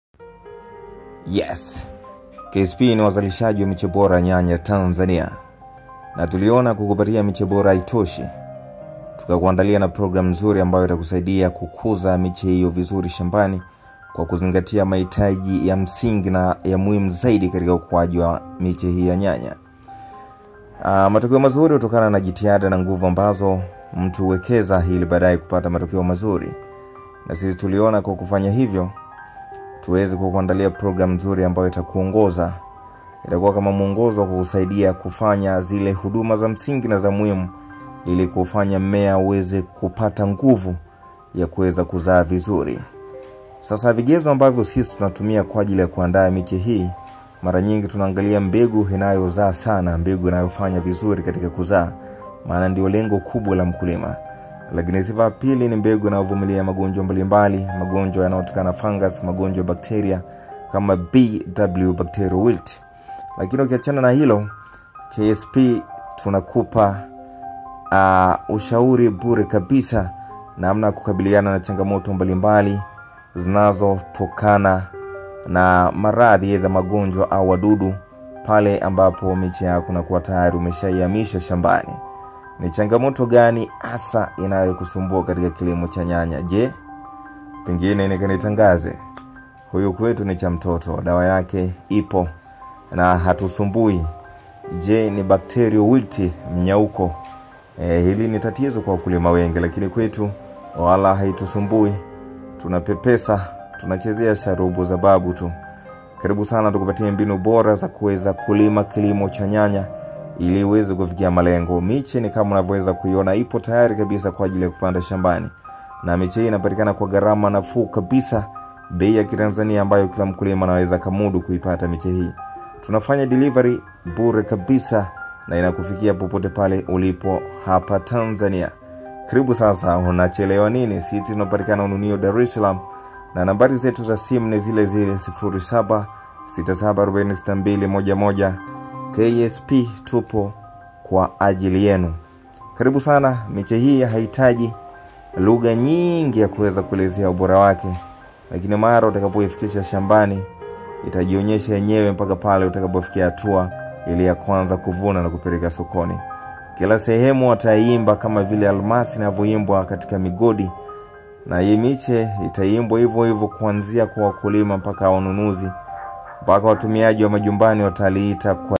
Tumekuandalia Miche Bora ya nyanya, Hoho na Pilipili , Unaanza kuvuna baada ya miezi miwili 🔥🔥🔥
Tsh 120 inatosha kukupatia Mche mmoja pamoja na program ya msimu mzima pamoja na kukutembelea shambani kwako.
Miche ya nyanya pilipili hoho nk

Tupo Ununio- Dsm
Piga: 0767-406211
Free delivery 🔥🔥🔥